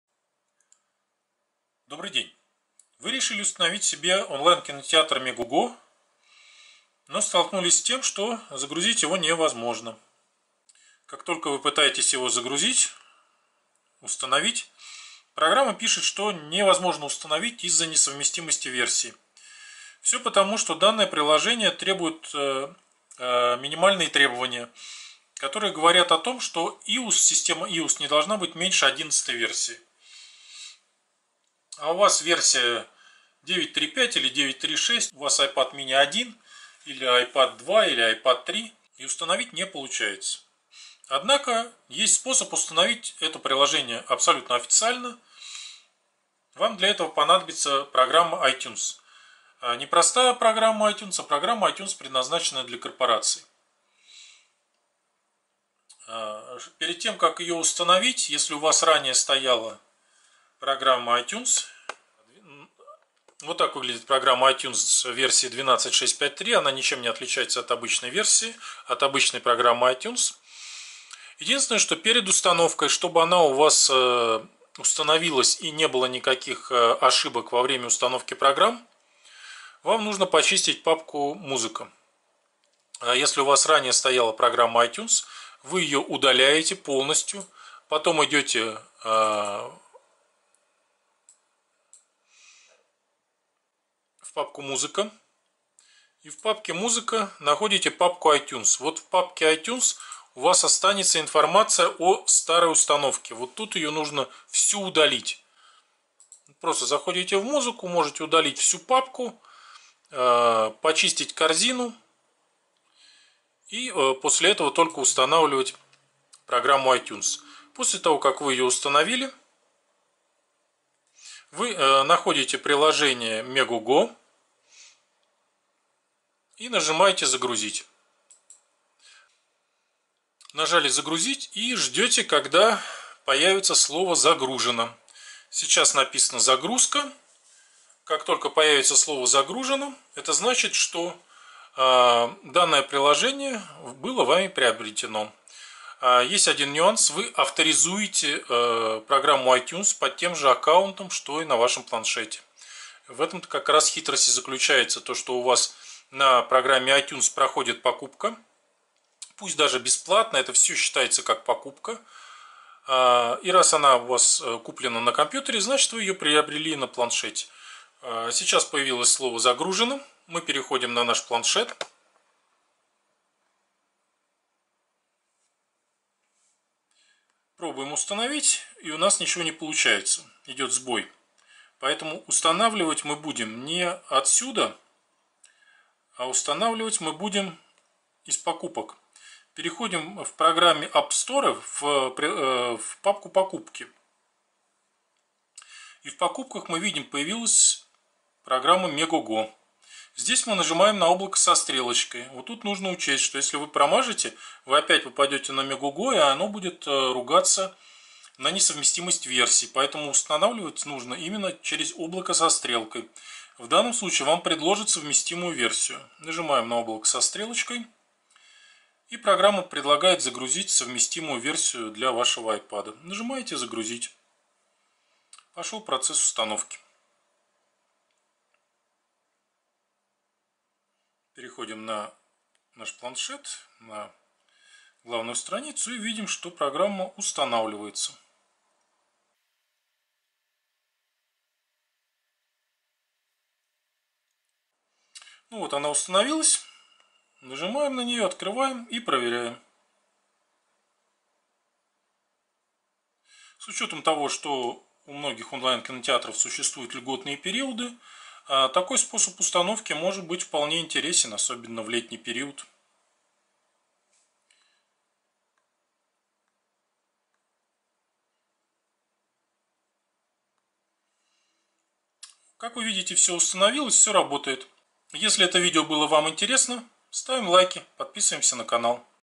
Онлайн кинотеатр Megogo устанавливаем на ipad из itunes 12.6.5.3
Если у вас старый ipad, такой как: ipad mini 1, ipad 2, ipad 3 и вы пытаетесь установить онлайн кинотеатр Megogo или какую-либо другую программу. Но во время установки программа выдает фразу - не совместимо с данным устройством. То вы можете попробовать установить через программу iTunes с версией 12.6.5.3
Данный способ позволяет установить множество программ на старые ipad с системой Ios 7,8,9,10,11 и т.д.
Вот ссылка откуда можно скачать программу iTunes

If you have an old ipad, such as: ipad mini 1, ipad 2, ipad 3 and you are trying to install Megogo online cinema or some other program. But during installation, the program outputs the phrase-not compatible with this device. Then you can try to install through the iTunes program with version 12.6.5.3
This method allows you to install many programs on old ipads with iOS system 7,8,9,10,11, etc.
Here is the link from where you can download the iTunes program

Si tiene un ipad antiguo como: ipad Mini 1, ipad 2, ipad 3 y está intentando instalar un cine en línea Megogo o algún otro programa. Pero durante la instalación, el programa emite una frase, no es compatible con este dispositivo. Entonces puedes intentar instalar a través del programa iTunes con la versión 12.6.5.3
Este método le permite instalar muchos programas en iPads antiguos con el Sistema iOS 7,8,9,10,11, etc.
Aquí hay un enlace desde donde puedes descargar el programa iTunes